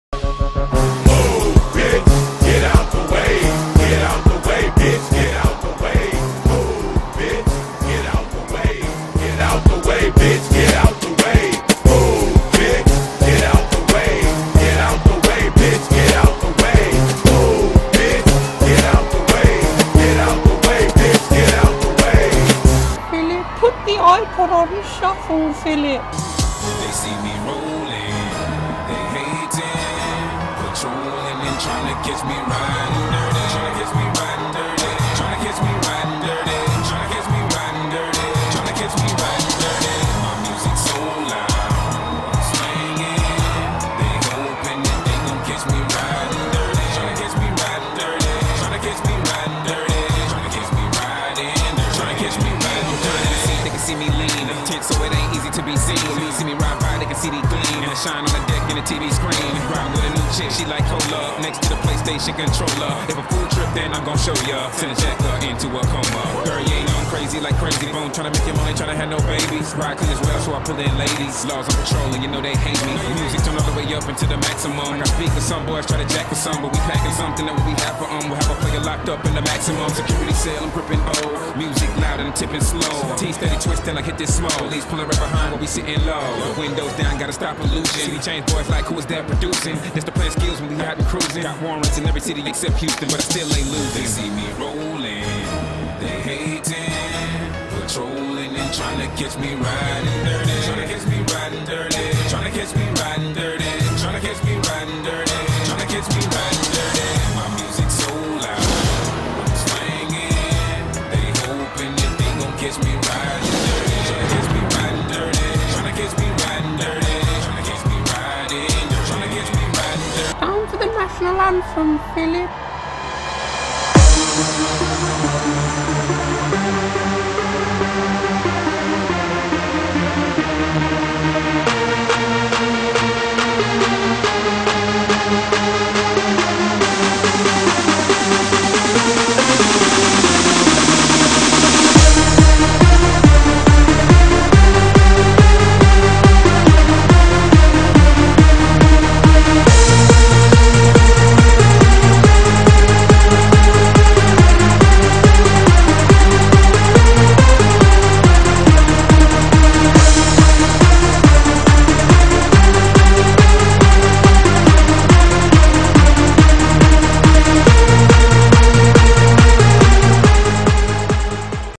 The Queen Rolling
We were lucky enough to have cameras inside the car that the Queen was in, on the way to the wedding of Prince William and Kate Middleton. Classy taste of music as she is rolling down to Westminster Abbey.